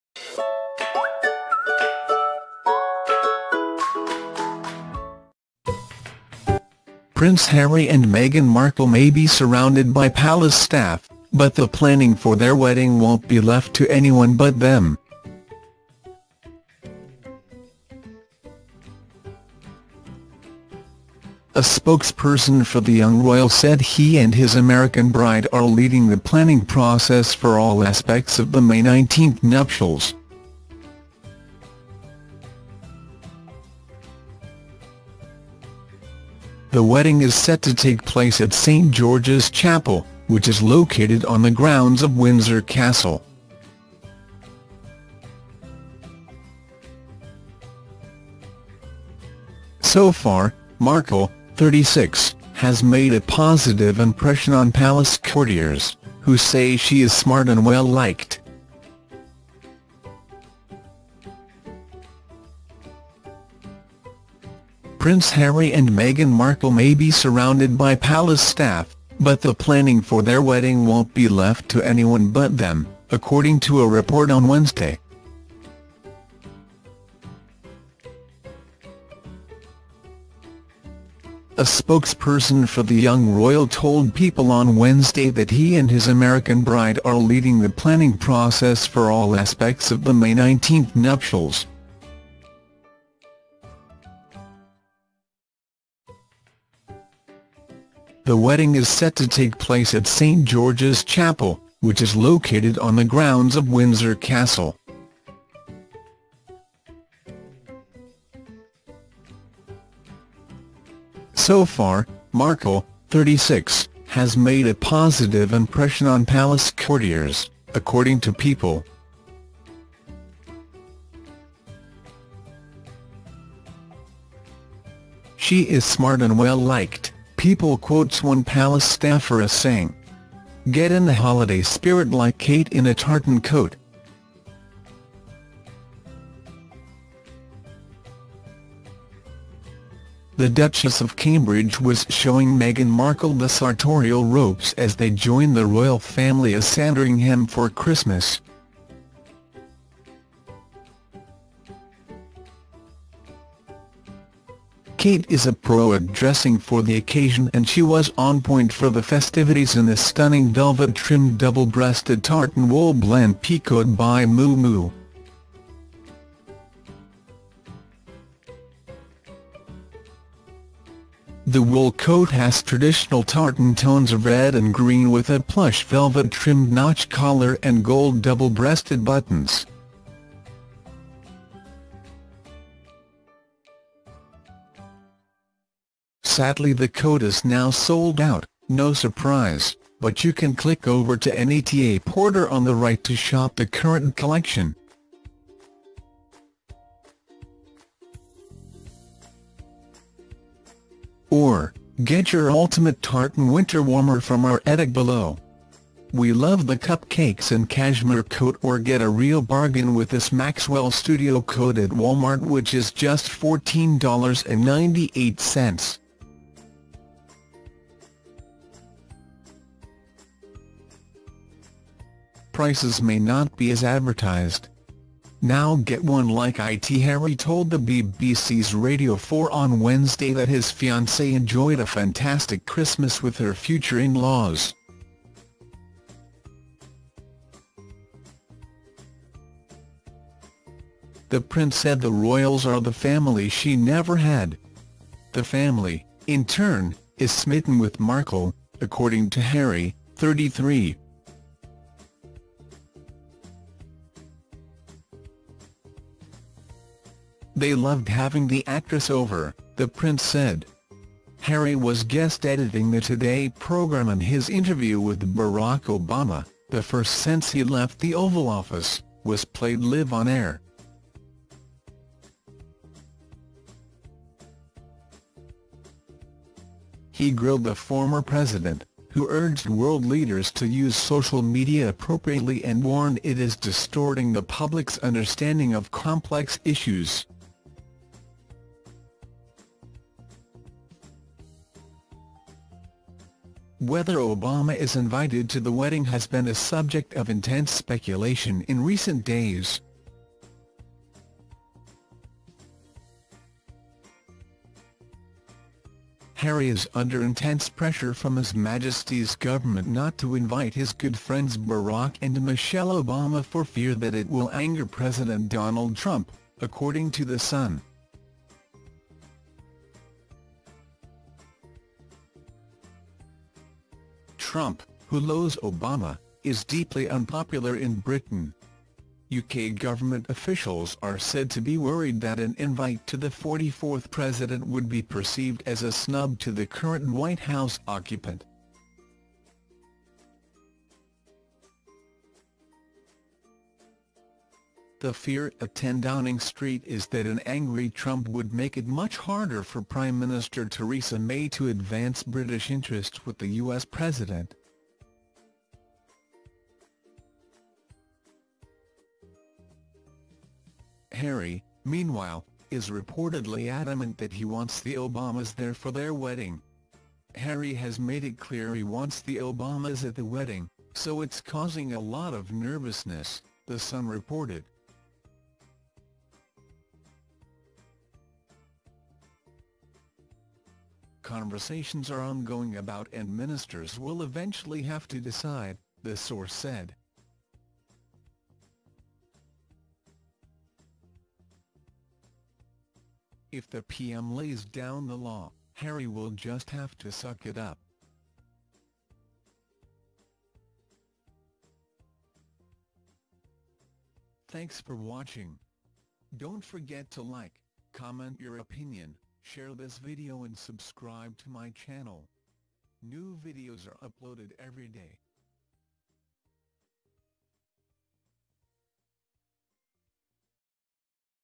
Prince Harry and Meghan leading the planning process for wedding Breaking News
Prince Harry and Meghan Markle may be surrounded by palace staff but the planning for their wedding wont be left to anyone but them A spokesperson for the young royal said he and his American bride are leading the planning process for all aspects of the May 19 nuptials The wedding is set to take place at St.

✖ Follow channel(S